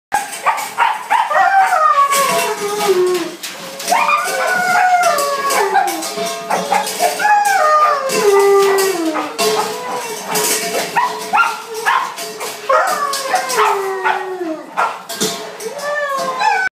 Sugar chases her food bowl in the crate
This is the new thing to do while waiting to go out in the morning